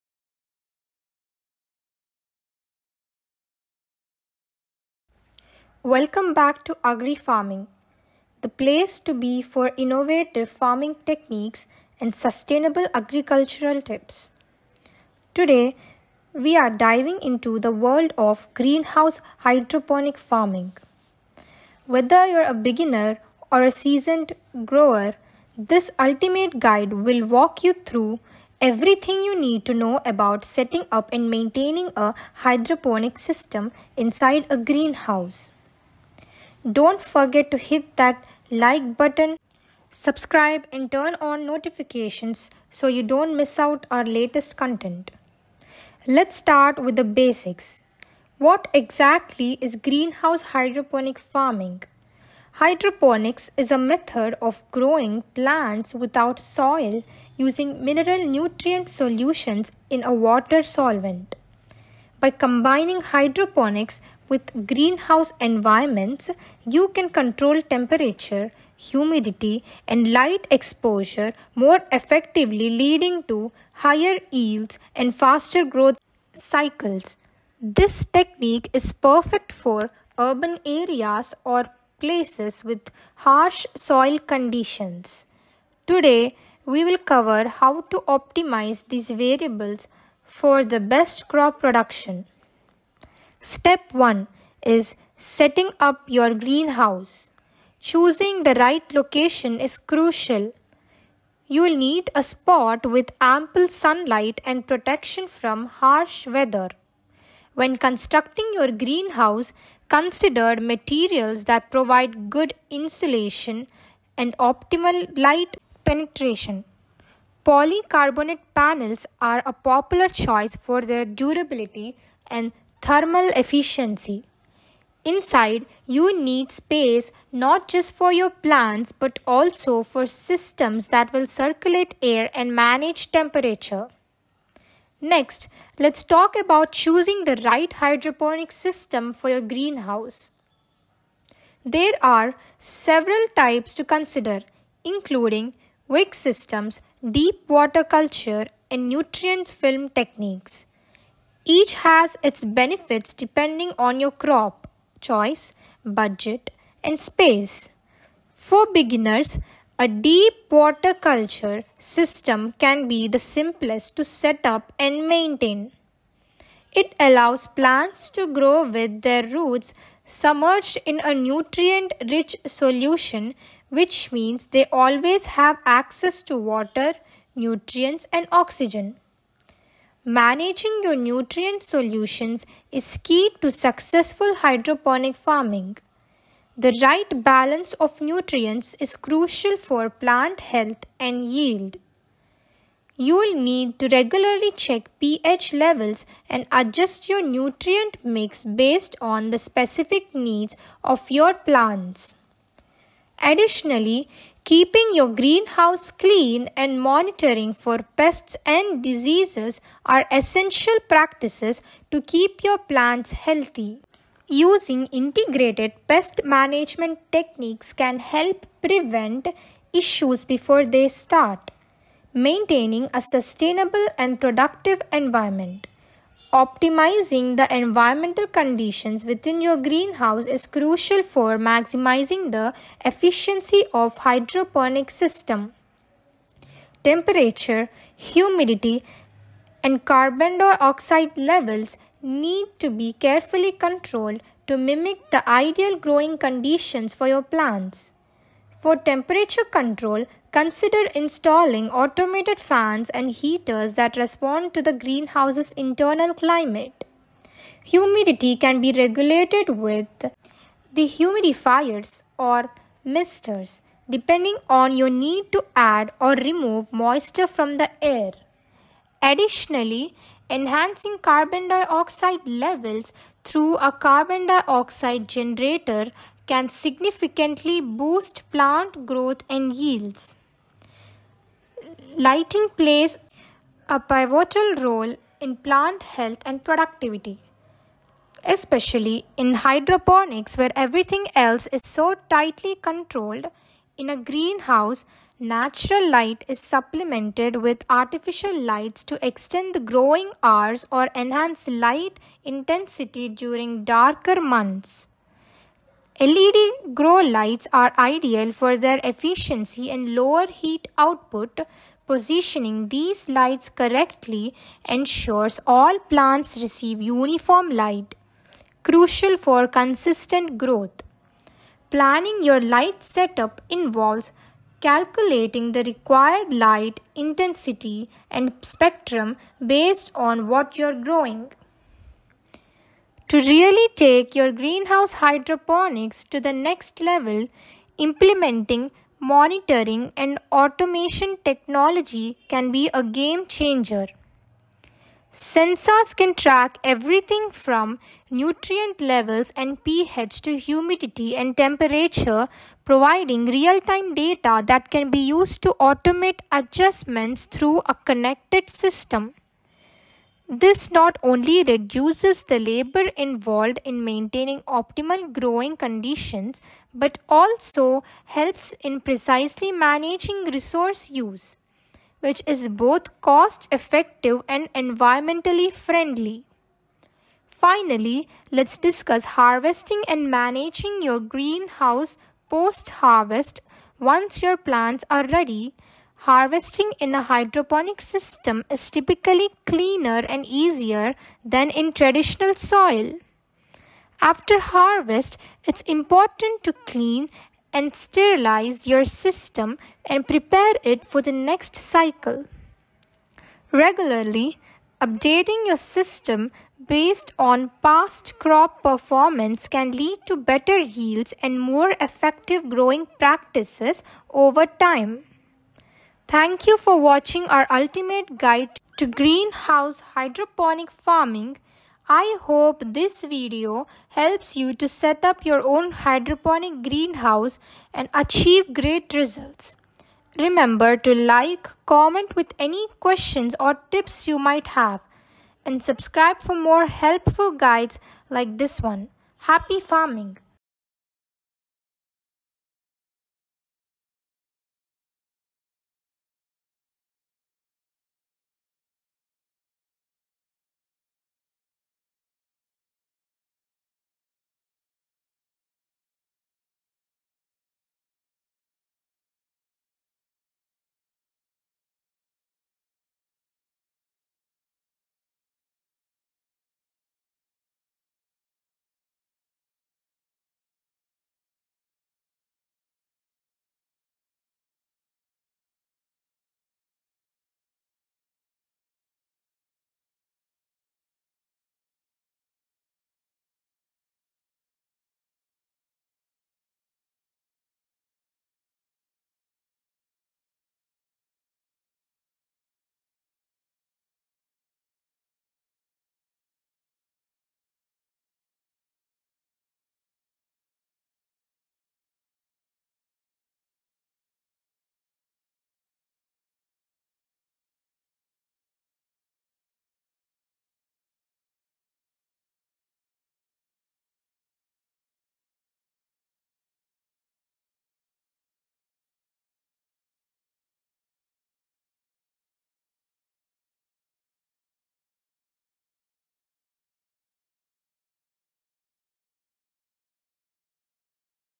Greenhouse Hydroponics | Subscribe | Support | Agri Farming
Dive into the world of hydroponic farming within a greenhouse setting with our latest video! Discover how integrating hydroponics into greenhouse cultivation can drastically increase crop yields, conserve water, and optimize plant health. We take you behind the scenes of a fully operational hydroponic greenhouse, showcasing the innovative techniques and technologies that make it all possible.

Learn about the essentials of hydroponic systems, from the setup and maintenance to the different methods like the Nutrient Film Technique (NFT), Deep Water Culture (DWC), and Aeroponics. Hear firsthand from seasoned hydroponic farmers and experts who share their experiences, challenges, and the benefits of growing crops in a controlled, soil-less environment.

Whether you're a hobbyist gardener or a commercial grower, this video will provide valuable insights into making your hydroponic greenhouse more productive and sustainable.